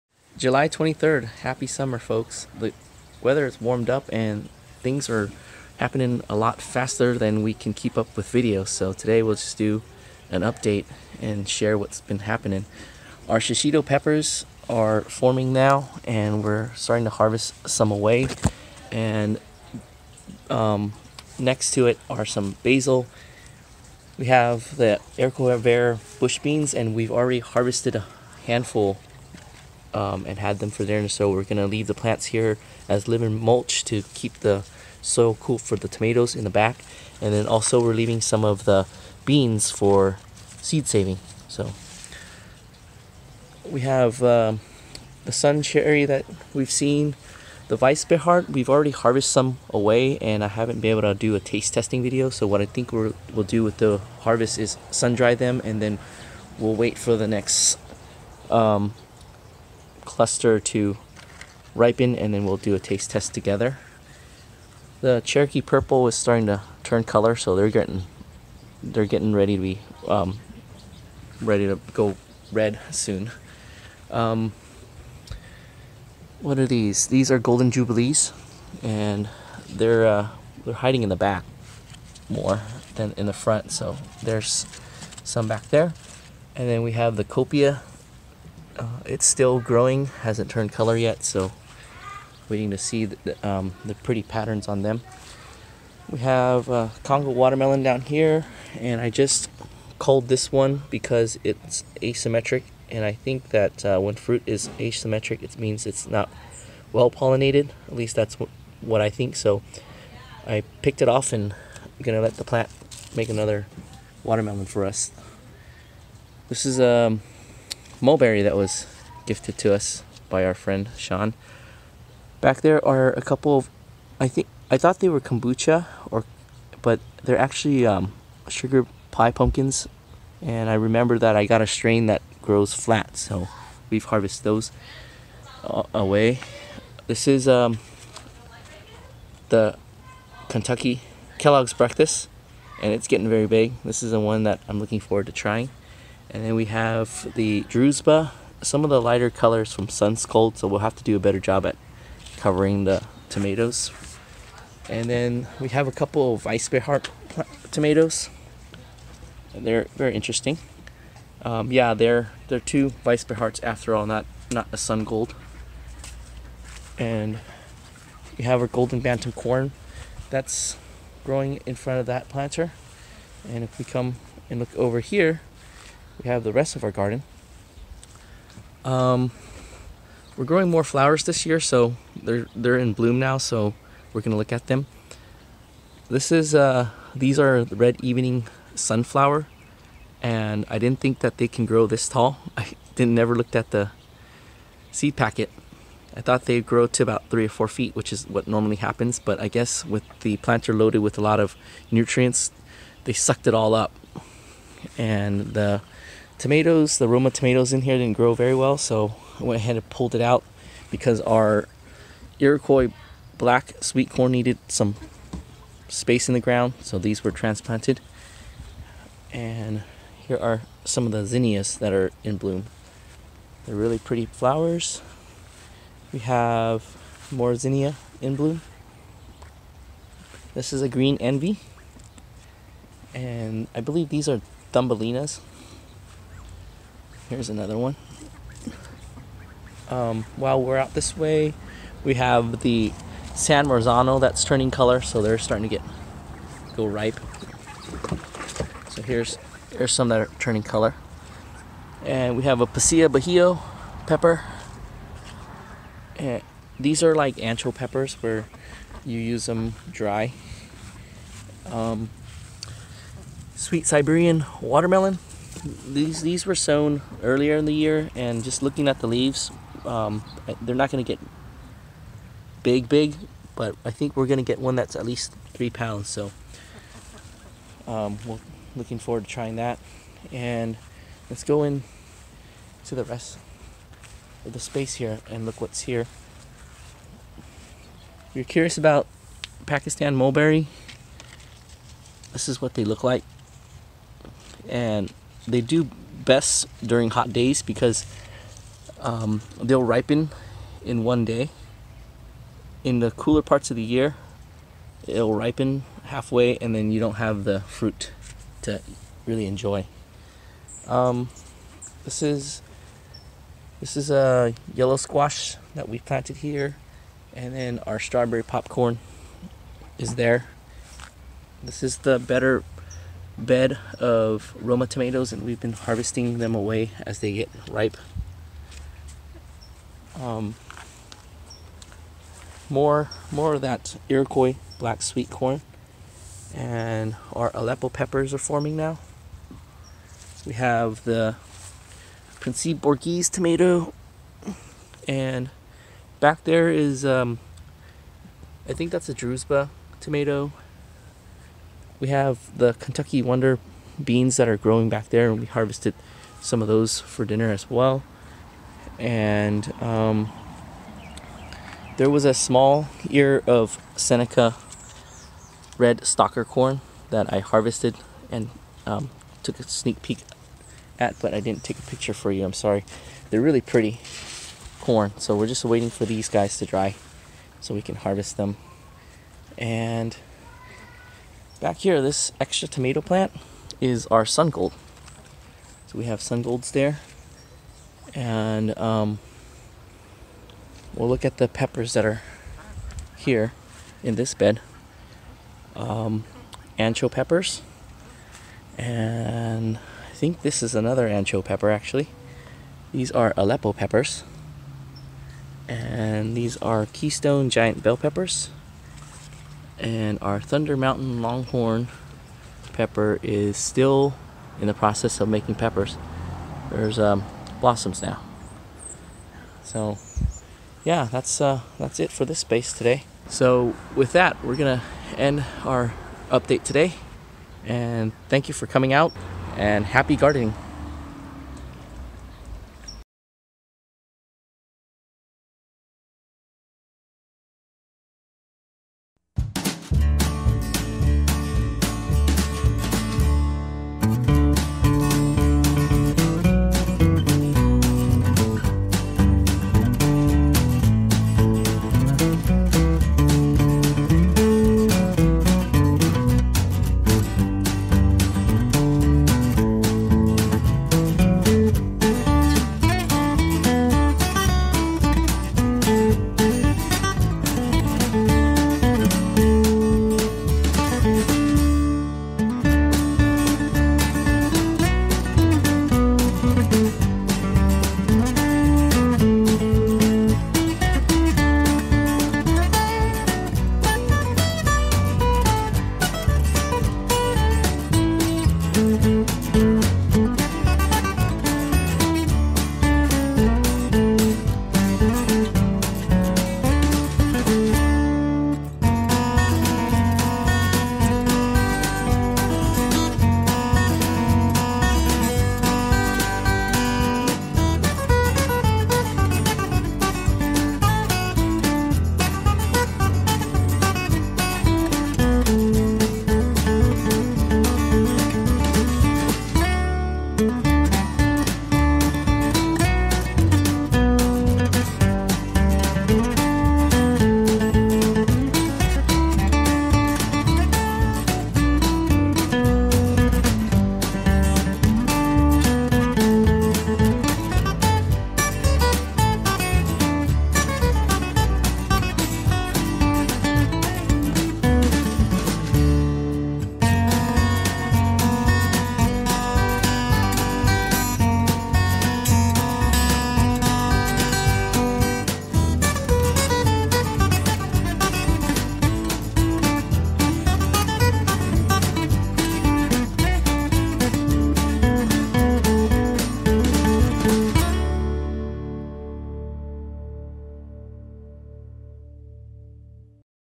2018 June 23 -- Garden Update
We have officially crossed over into summer. Days are getting much warmer. Which means that the plants are growing at a much faster clip. These warm days means the return of day time mosquito. Looks like they are here to stay. At the bonus clip where I am removing the roma tomatoes and transplanting corn, you can also see me testing out some new gardening gear-- pants and long sleeve sun reflecting shirt. I would much rather garden in t-shirt and shorts but it gets a bit annoying having to apply bug spray all the time.

Zone 10b / San Gabriel Valley / Los Angeles / Southern California / USA